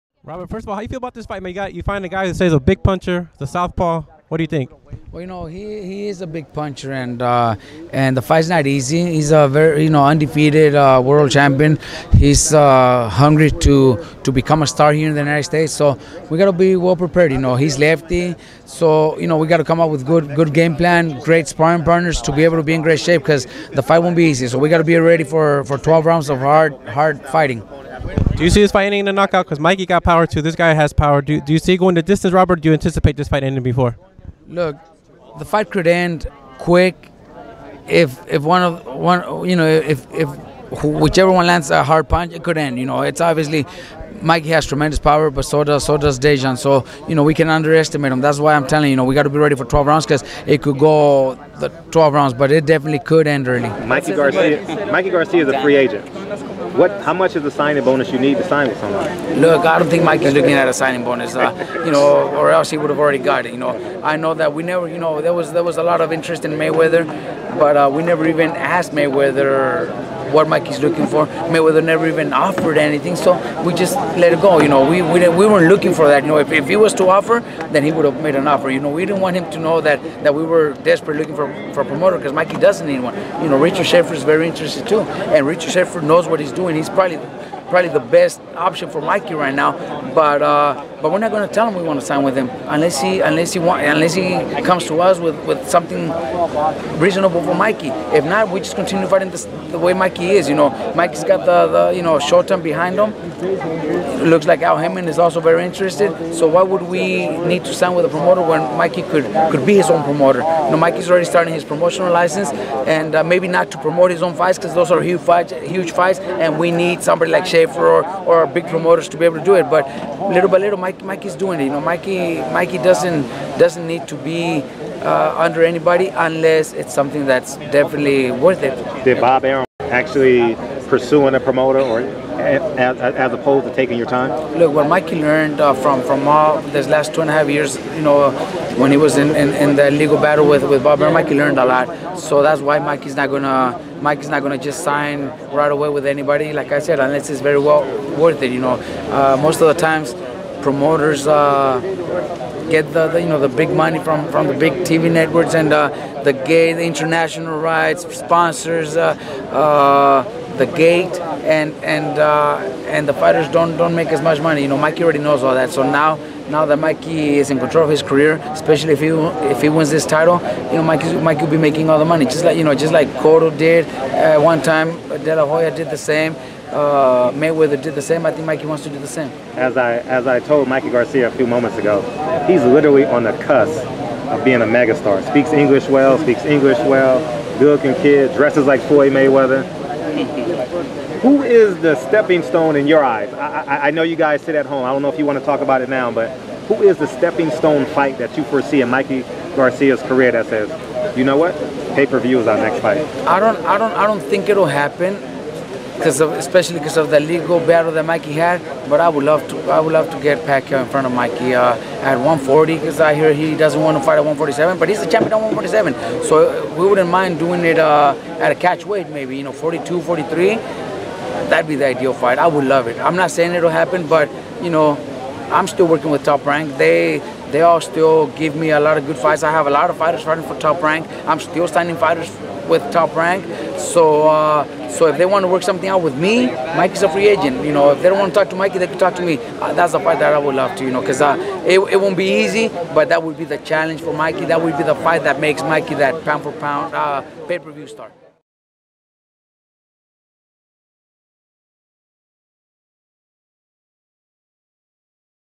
ROBERT GARCIA PREDICTS MIKEY GARCIA COULD STOP ZLATICANIN EARLY; EXPLAINS PROMOTIONAL STATUS
"He is a big puncher...the fight's not easy...he's an undefeated world champion...we gotta be well-prepared...we gotta come up with a good game plan...we gotta be ready for 12 rounds of hard fighting...the fight could end quick...Mikey has tremendous power, but so does Dejan," stated world-class trainer Robert Garcia, who talked about Mikey Garcia's upcoming title clash with WBC lightweight champion Dejan Zlaticanin. Check it out!